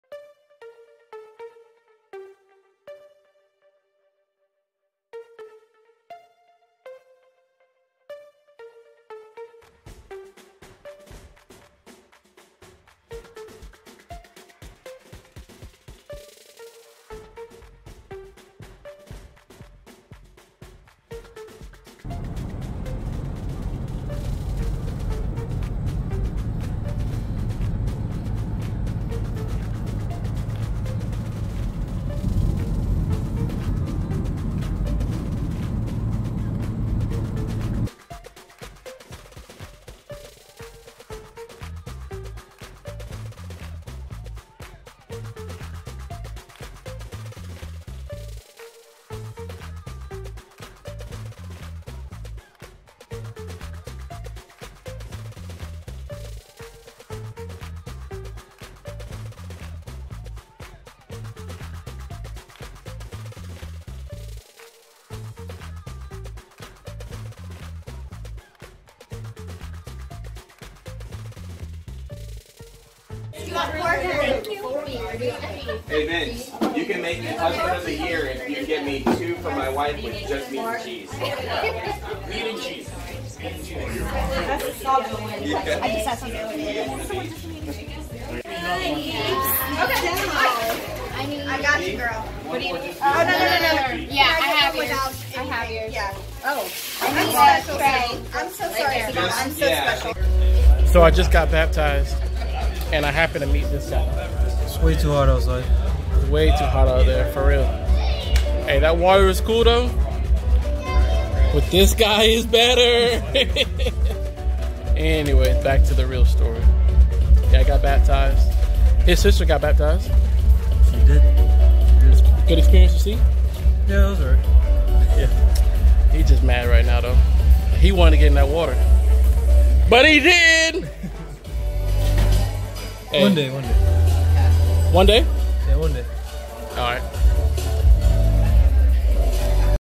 A Day In The Life Pt. 2
Follow us during a weekend in the life of the Randolph's!